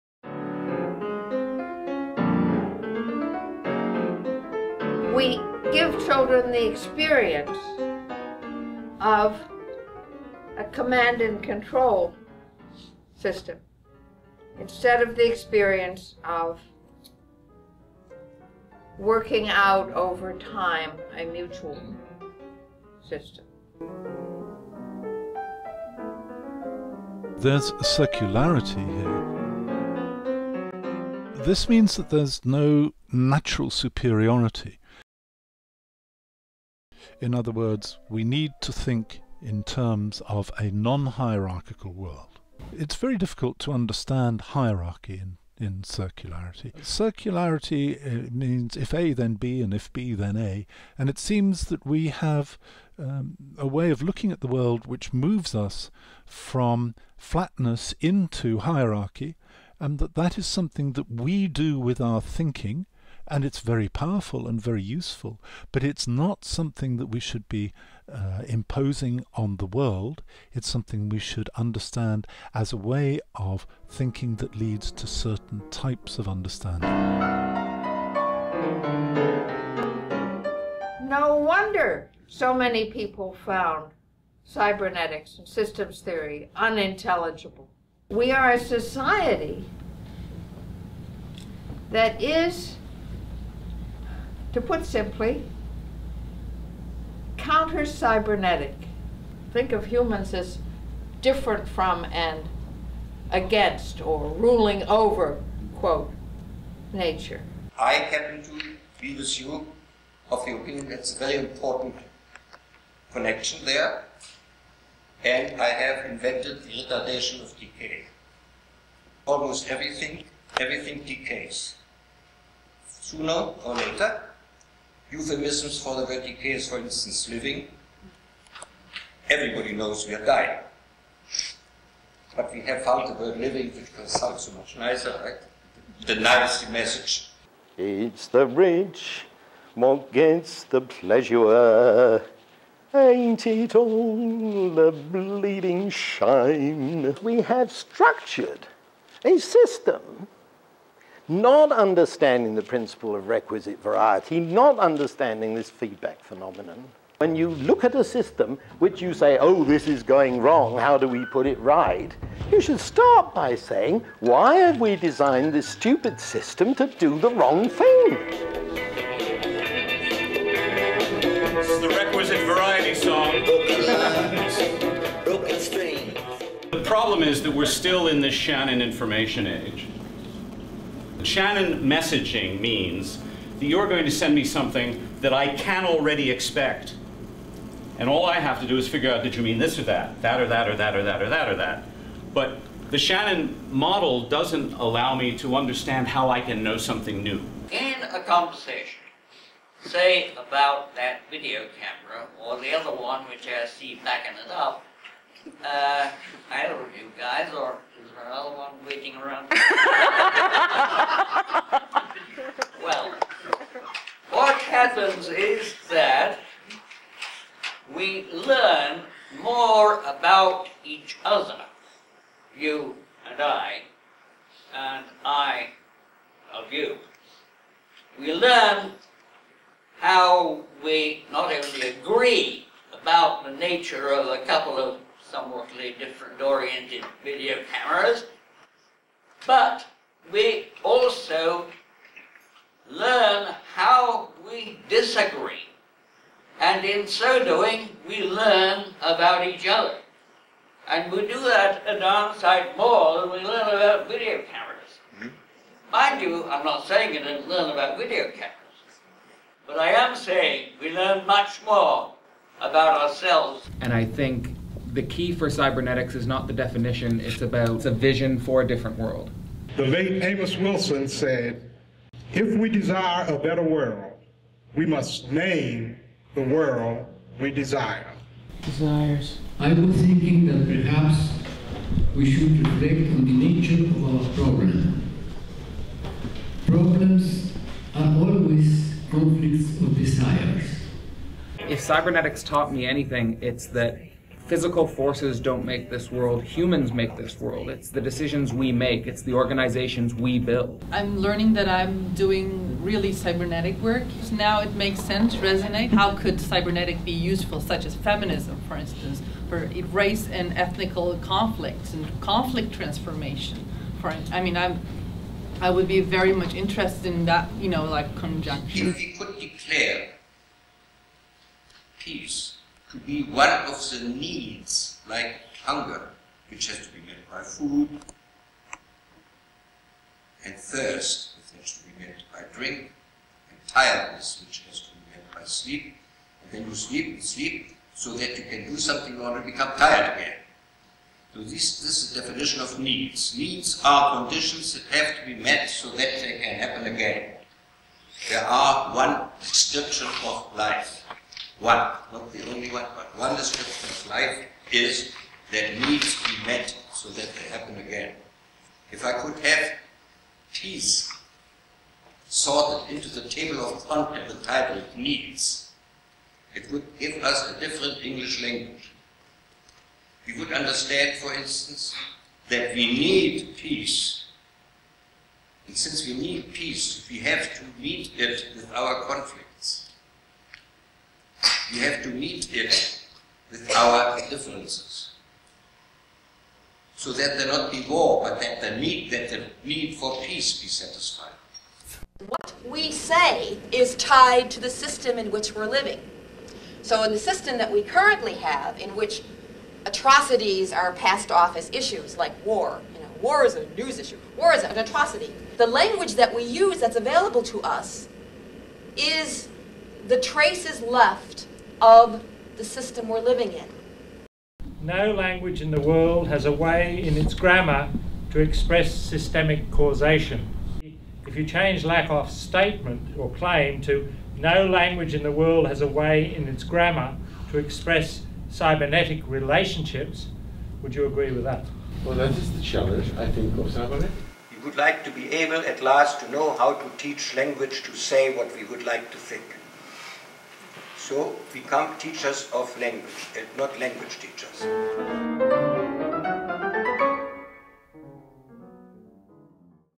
A Cybernetic Challenge
In this montage a group of people talk (language) about a variety of cybernetic ideas, notions and concepts.